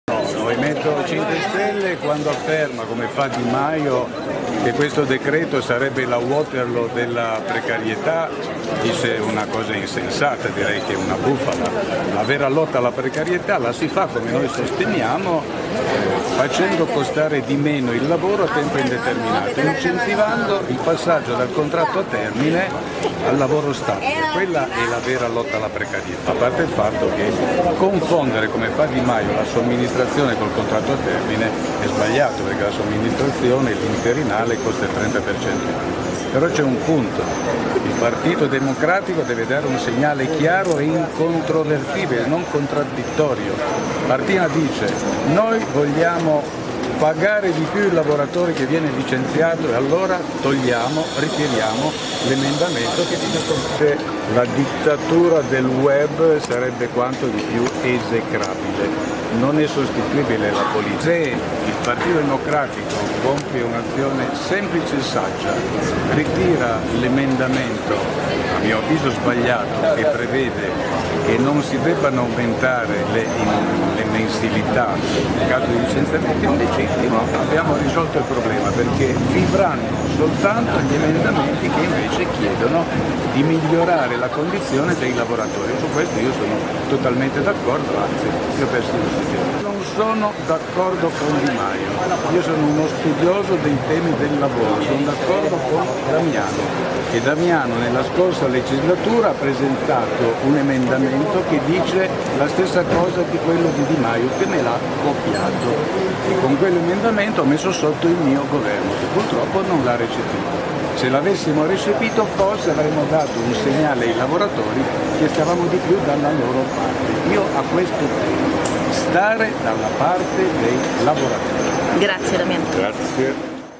DLDignità, Damiano: "Di Maio mi ha copiato e ha messo sotto il governo"
POLITICA (Roma). Cesare Damiano si ferma con i cronisti prima di entrare al Nazareno per la riunione della direzione del PD: "Se come dice Martina l'intenzione del PD è aumentare le indennità, non vedo perché si sia presentato un emendamento che dice il contrario. Un errore cui si può porre rimedio lasciando vivere gli altri. Dobbiamo essere chiari e non contraddittori e dalla parte dei lavoratori. Il movimento 5 stelle quando afferma che questo decreto sia la Waterloo è una bufala. La vera lotta di fa facendo costare di meno il lavoro a tempo indeterminato. Confondere la somministrazione con il contratto a termine è sbagliato. Il PD deve dare un segnale chiaro e inconvertibile. La dittatura del web sarebbe quanto di più esecrabile e non sostituibile alla politica. Se il PD compie un'azione semplice e saggia, ritira l'emendamento, abbiamo risolto un problema. Non sono d'accordo con Di Maio. Sono uno studioso. Di Maio mi ha copiato, e non l'abbiamo recepito. Io voglio che si sappia: il PD è dalla parte dei lavoratori". (Luigi Pellicone/alaNEWS)